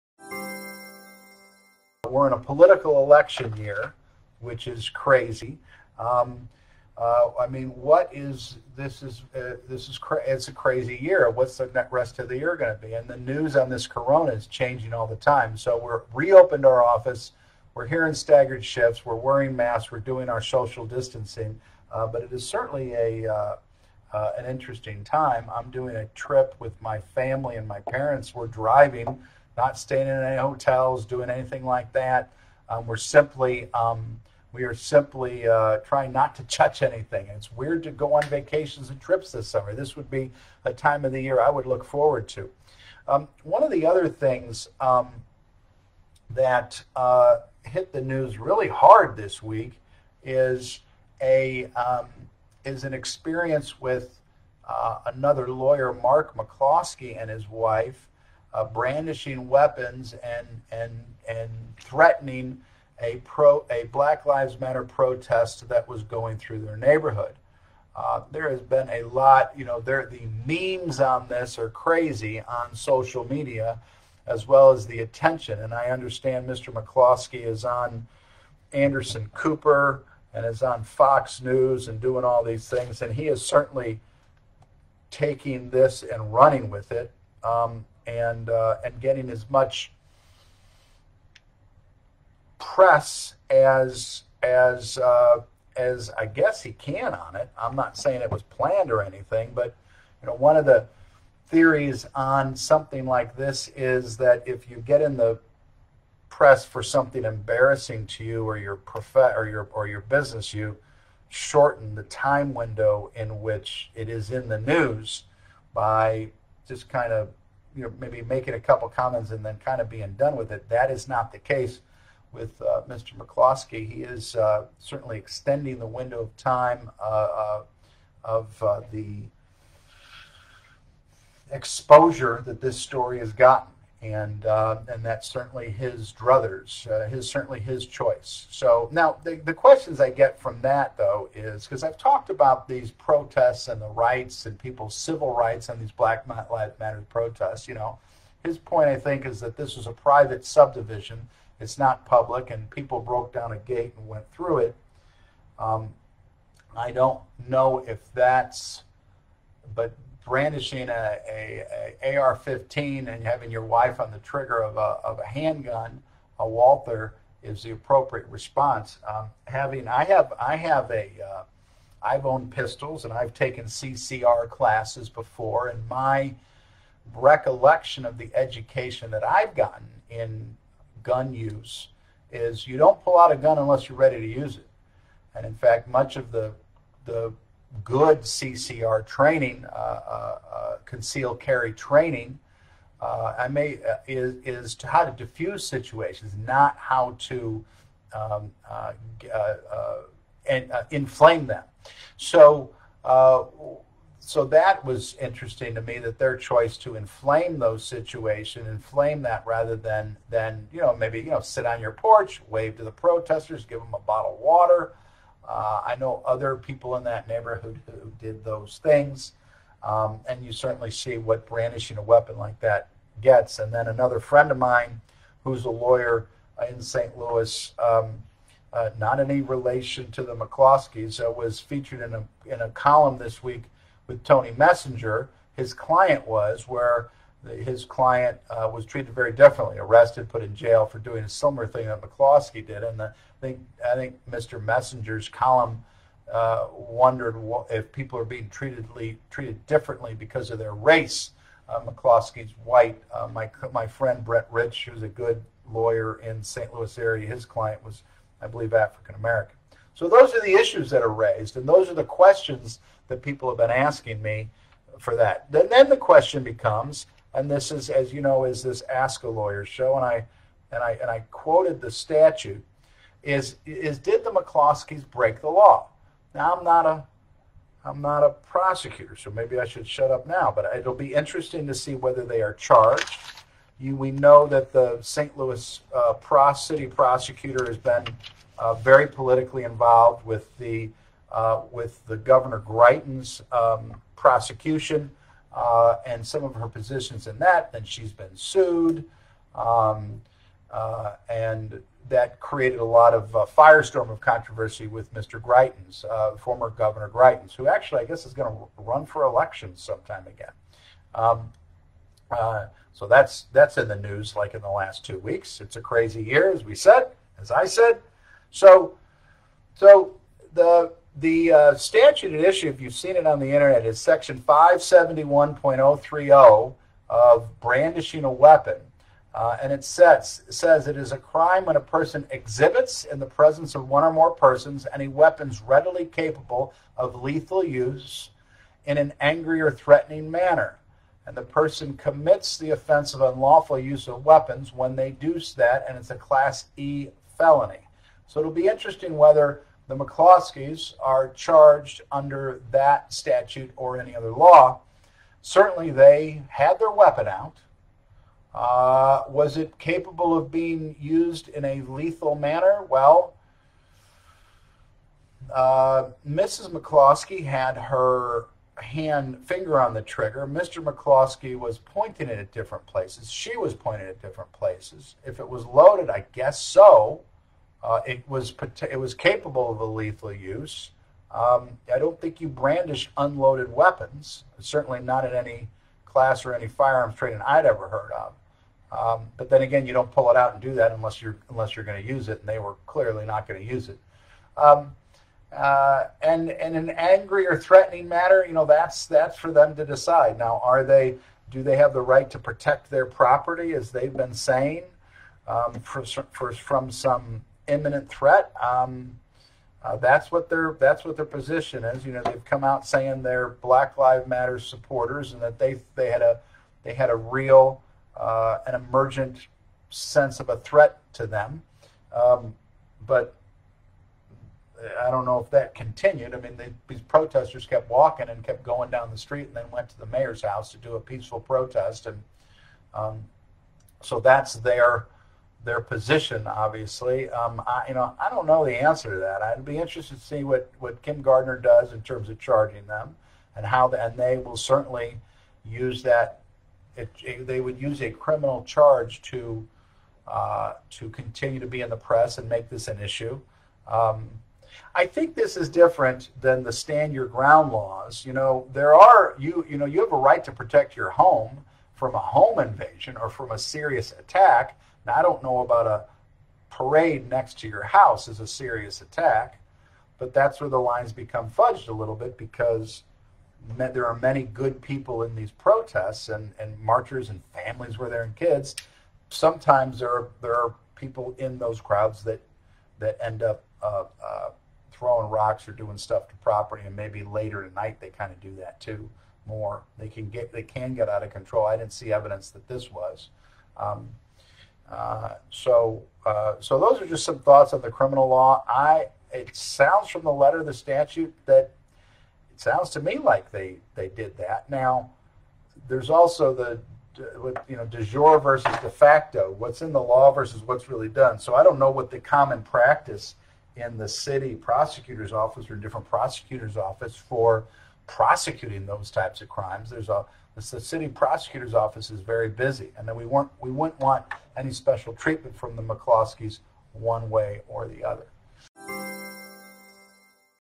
Real Lawyer Reacts to McCloskeys Brandishing Firearms at Protesters - Was It Legal?
Real Lawyer Reacts to the McCloskeys Brandishing Firearms at Protesters - What Are Their Rights? Was It Legal?

Gary Burger of Burger Law talks about the incident when a St. Louis Couple Pointed Guns At Protesters.

** Update: Charges have been filed against the St. Louis couple who brandished guns at protesters. A husband and wife caught on video brandishing firearms at Black Lives Matter protesters outside their St. Louis home were charged with a felony.

Mark and Patricia McCloskey were each charged with felony unlawful use of a weapon by St. Louis Circuit Attorney Kimberly Gardner. The couple forfeited their weapons to police earlier this month after a warrant was issued for their seizure.

St. Louis Circuit Attorney Kim Gardner, who filed the charges against the McCloskeys, did not order the couple to surrender or be arrested. Instead, as part of Gardner’s reformist approach to reducing incarceration for low-level crimes, she issued summonses and said she would consider them for a diversion program, which would enable the charge to be dismissed if counseling or another remedial course were completed.